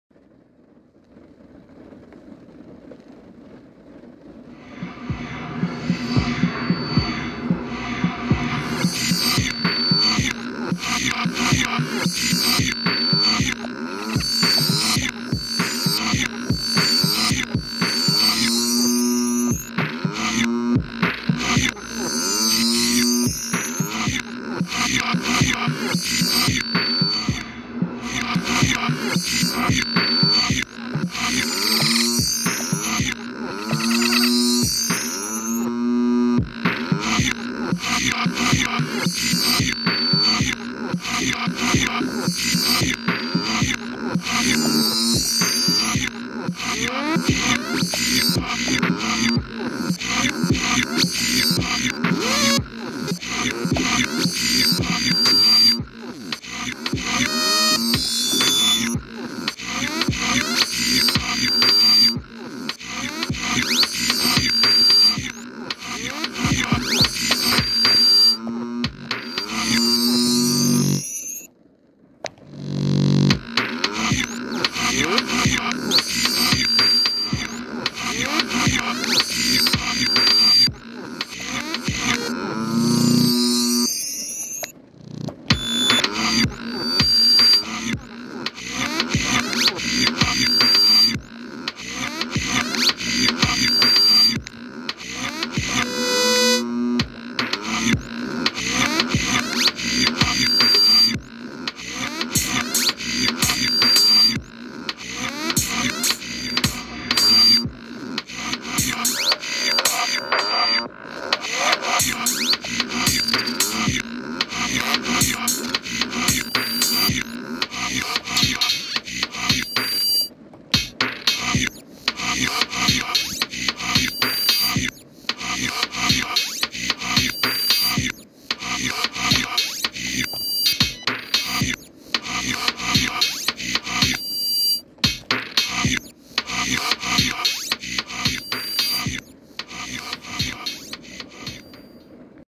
Korg Volca Sample Good Morning Madness
I made this quickly, just minutes before clocking in for work. hence the title! Just a little Korg Volca Sample Sample!